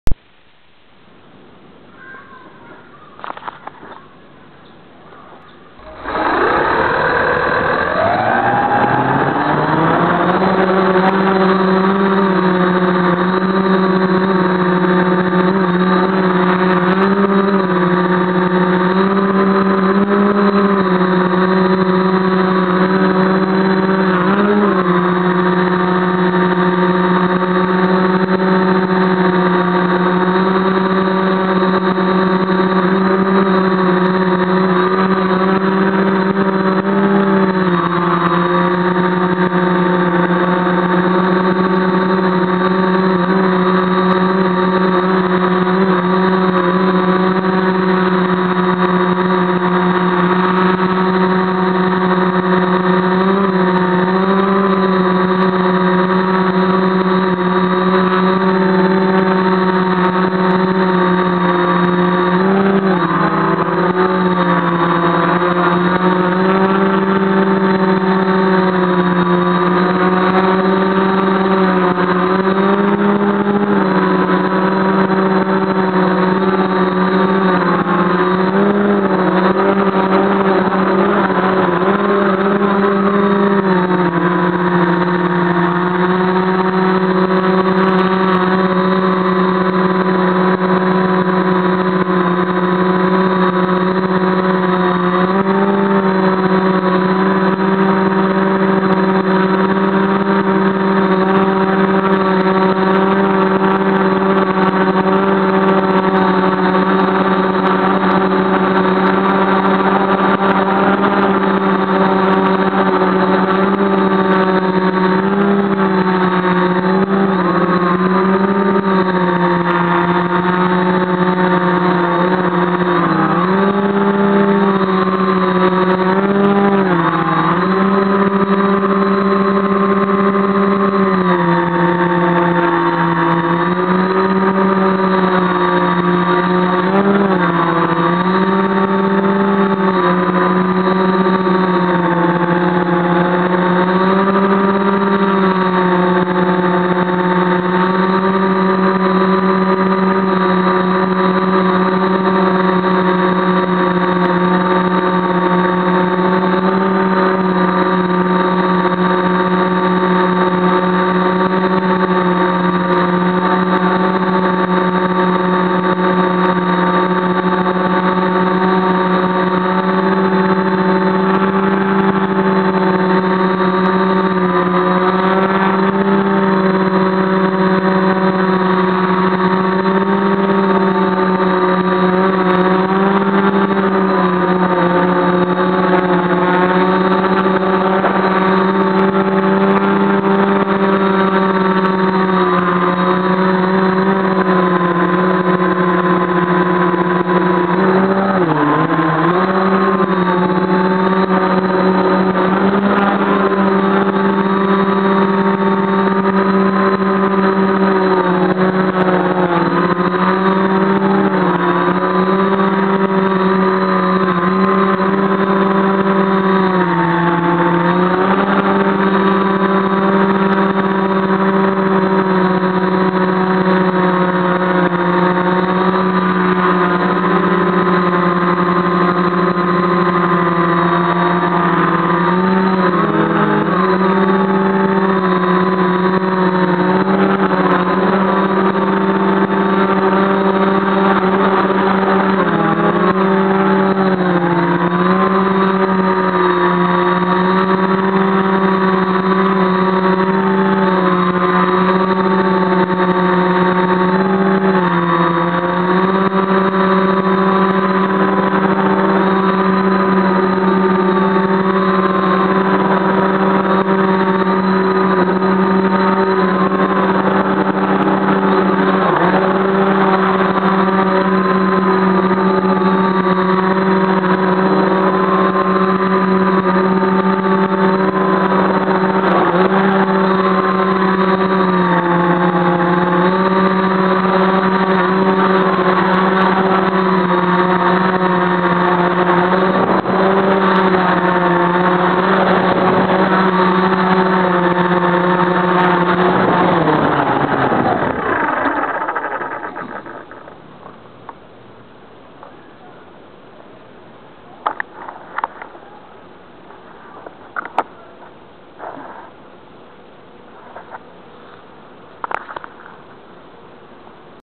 APM 2.5 yaw issues with TBS style frame, pid tuning but need independent motor access.
A good improvement may be a centre point indicator on the APM 2.5 cases. Also a peizo batt warning because with PMU the 2.5 platform is heavy as heavy gets as far as FC's.
The non symmetrical nature of the splayed frog frame is making rotations very unstable but they get better with fc position so back and forth might get that fixed. May spend more time tuning pods but need to get 11" props because the 930's are at their max rpm to spin the 1047's and not enough lift for punching the throttle on fast decents it seems.